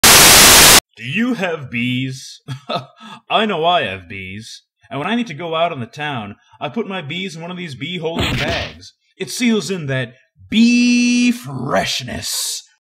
Classic Bee Holding Bags Advert
Back in 1956 this ad used to run on NBC and CBN as an advert for BeeCorp who went out of business in 1957. Bee Holding Bags have long been out of print and if you manage to find one, just know they were worth upwards to 25,000 dollars.